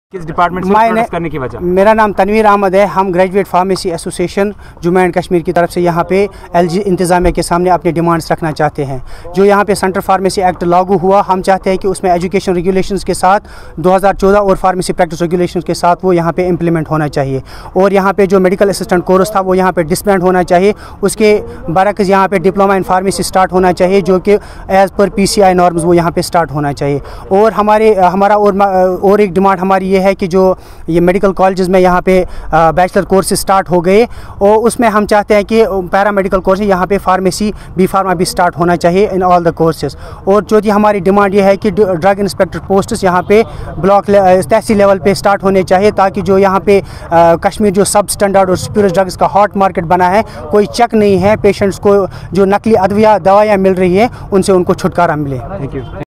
J&k Pharmacy Graduates Association stage silent protest in press enclave srinagar demands implimentation of Central pharmacy Act- 1948. Urges Administration to recruite pharmacy graduates in all health care institutes